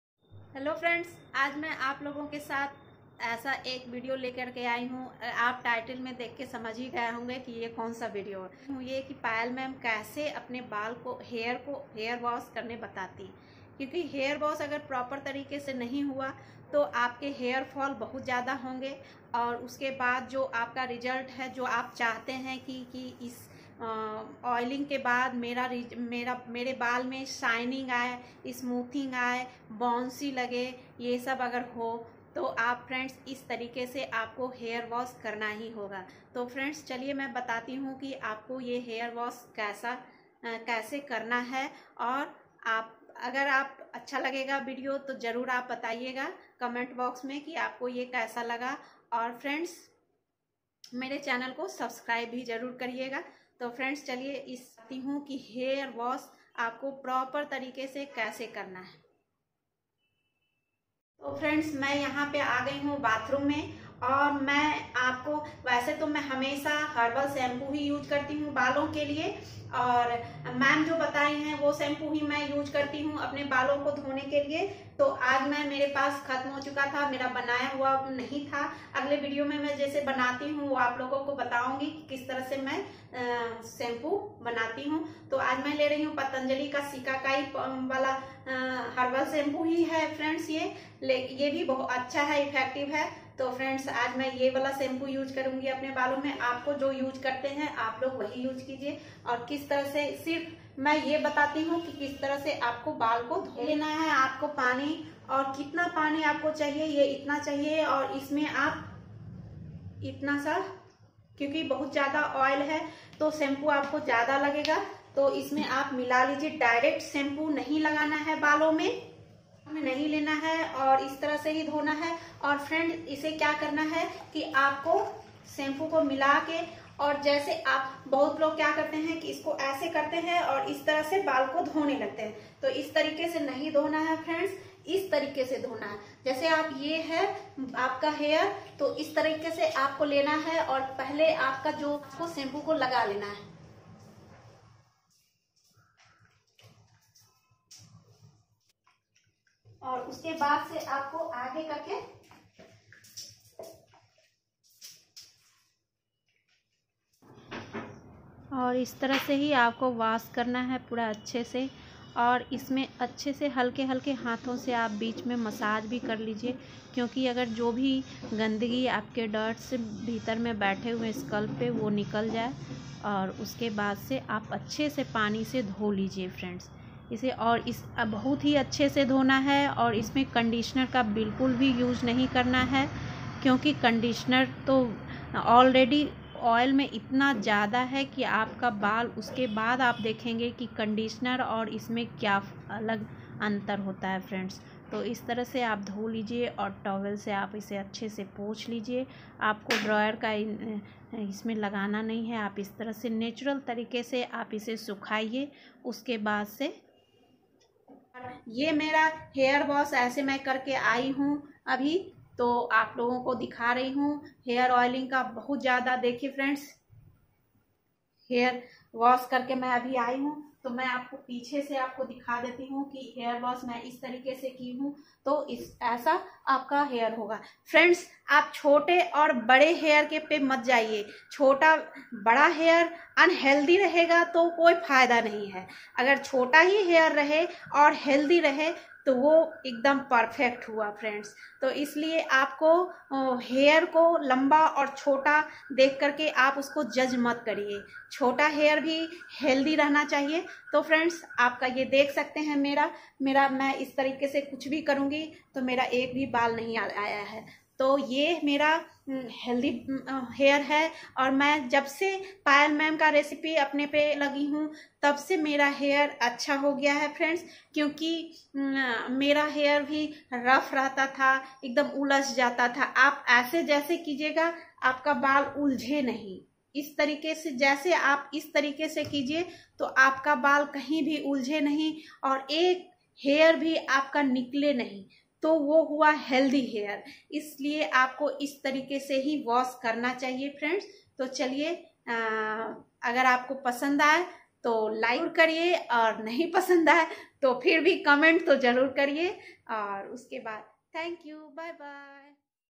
Hairwash after heavy hair oiling👍👍
hello friends heavy oiling ke baad proper tarike se hair wash kaise karen